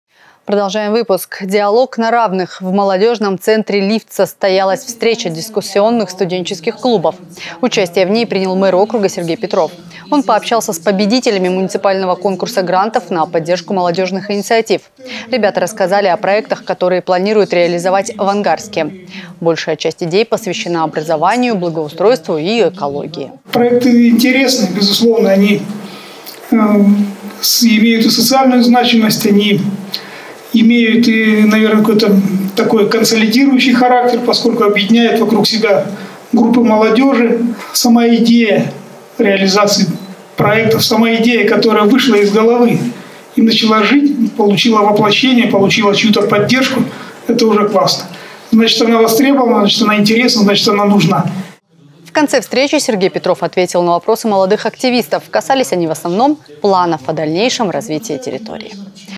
Диалог на равных
Мэр АГО обсудил с ангарской молодежью развитие родного города. Молодые люди стали авторами креативных социальных проектов.